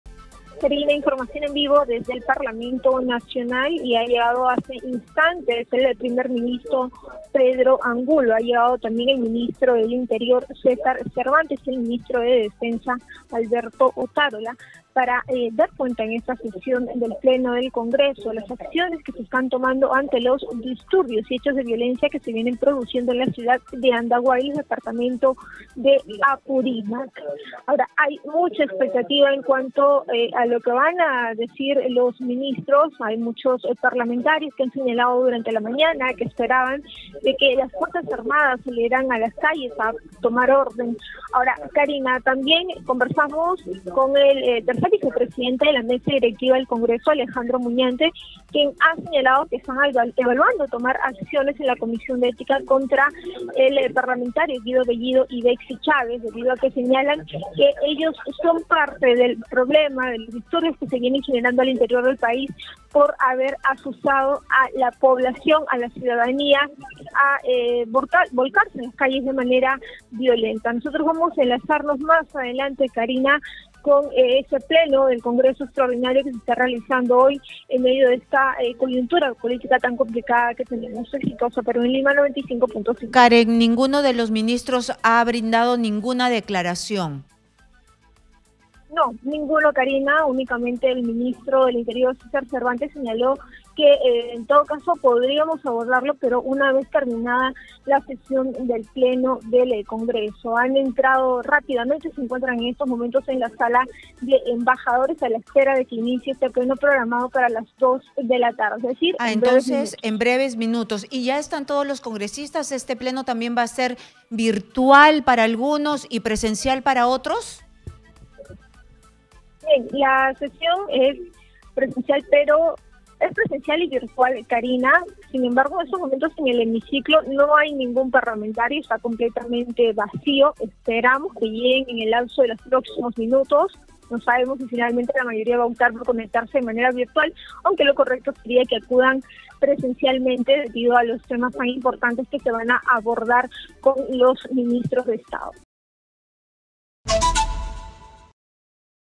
🔴🔵Premier junto a los ministros de Defensa e Interior llegan al Congreso de la República
El presidente del Consejo de ministros, Pedro Angulo en compañía de los ministros de Defensa e Interior, Alberto Otárola y César Cervantes llegaron a las instalaciones del Congreso de la República.

Exitosa Perú
"La voz de los que no tienen voz"

Corporación Universal